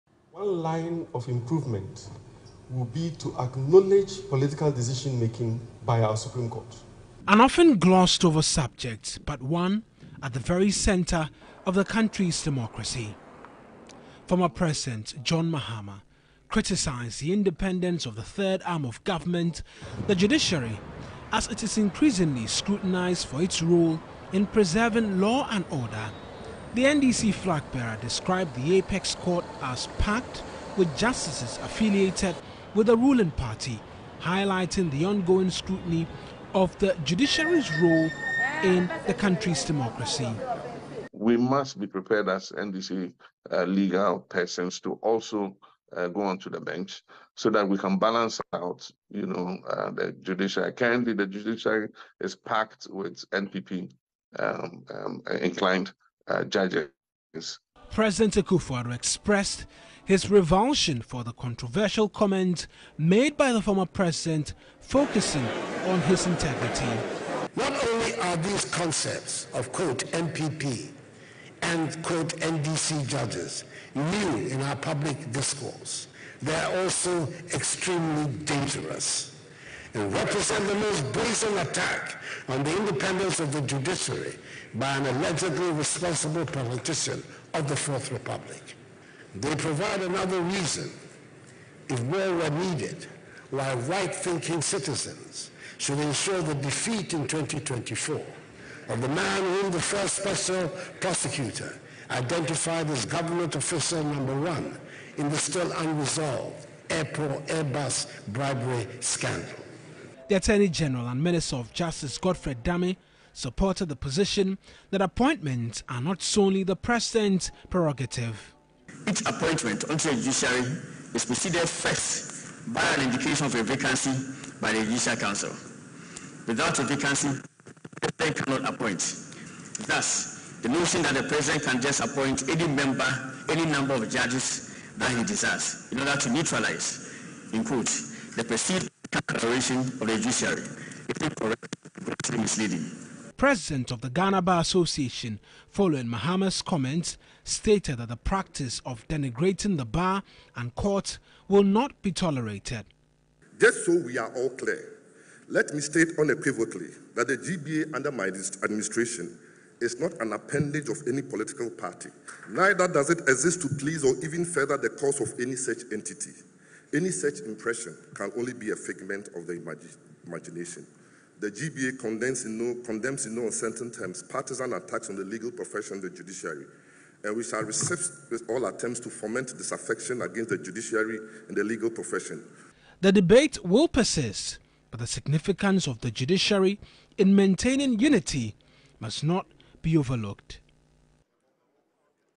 Mahama judiciary comment: Akufo-Addo chides predecessor for extremely dangerous and brazen attack
"Extremely dangerous and represents the most brazen attack on the judiciary”

This is the president's verdict on John Mahama's promise to even out the justices in the country's supreme court.

Addressing the 23rd bar conference in Cape Coast he urged Ghanaians to reject the NDC solely based on that comment.